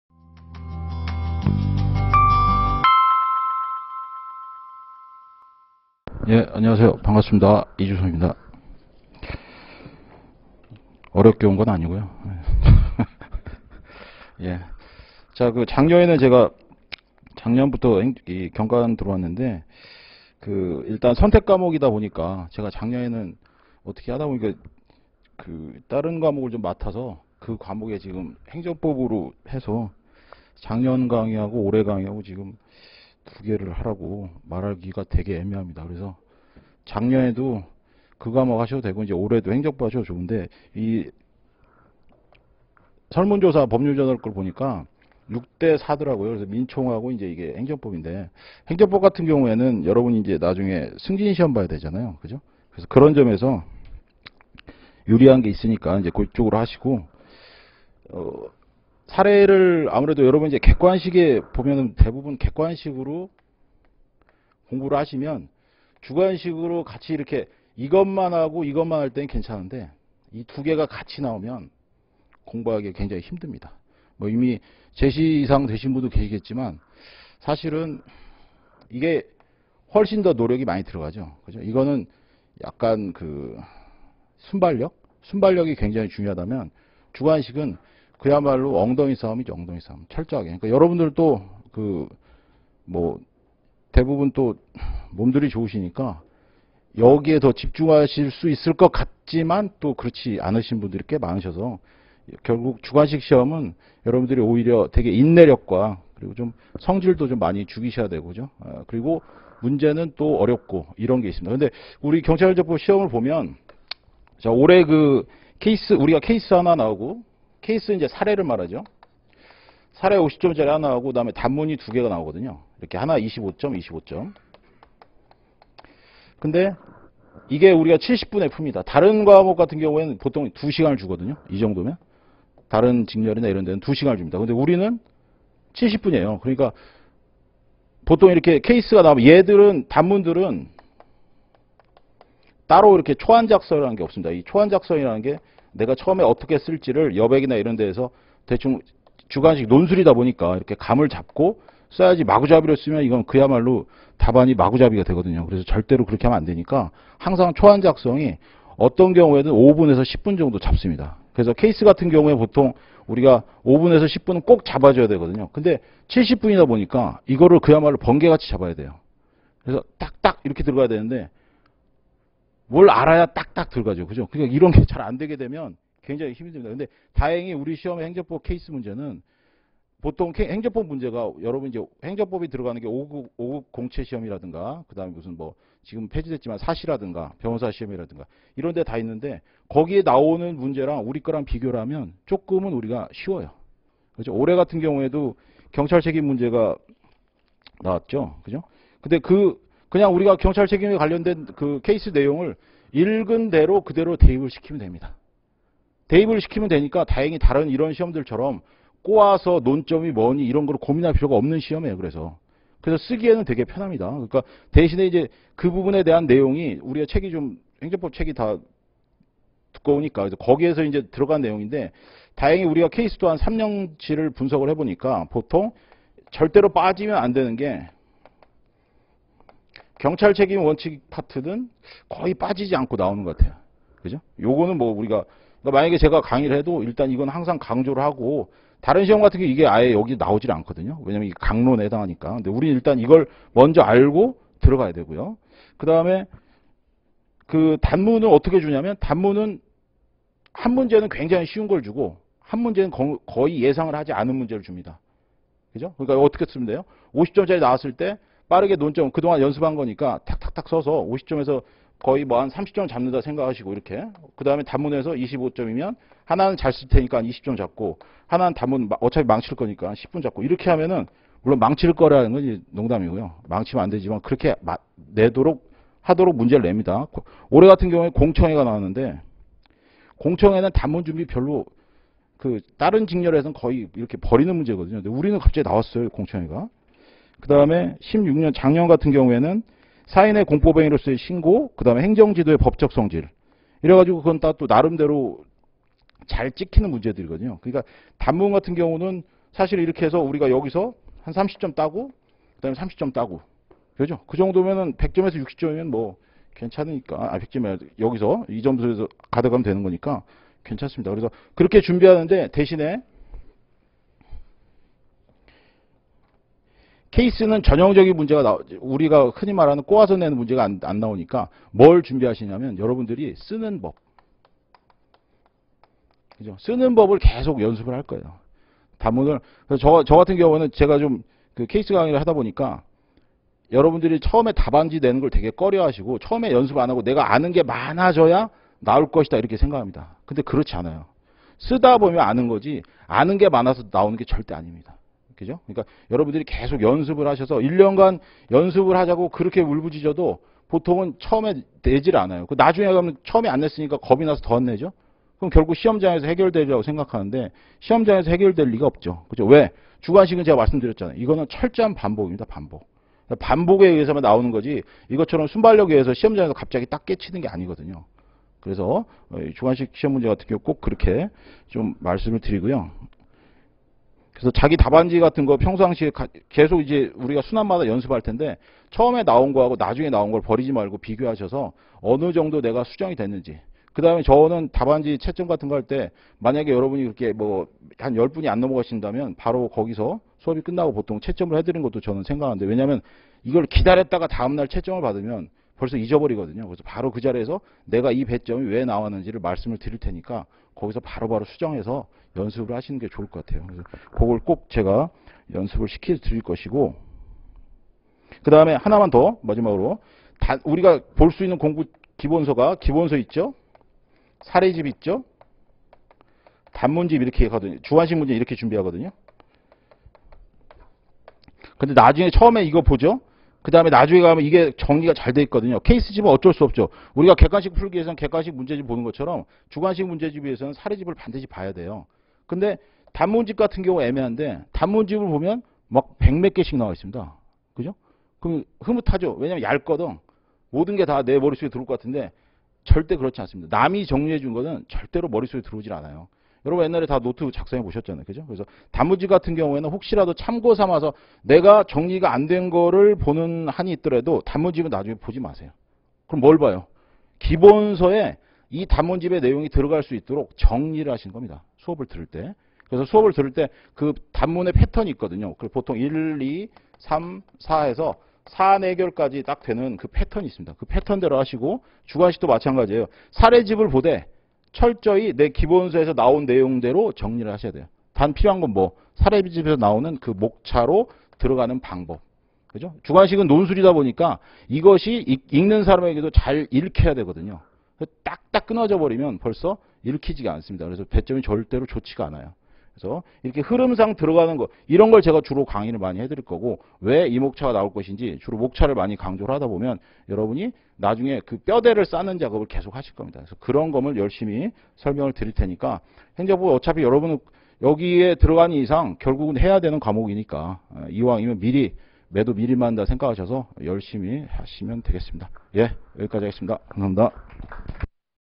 이주송(행정법)- 제68기 경간부 시험 합격을 향한 도전! 합격의법학원 설명회
합격의법학원 신림캠퍼스에서 10월 21일 진행된 법학원 2019년 68기대비 경찰간부 종합반 설명회[17年10月] 과정입니다.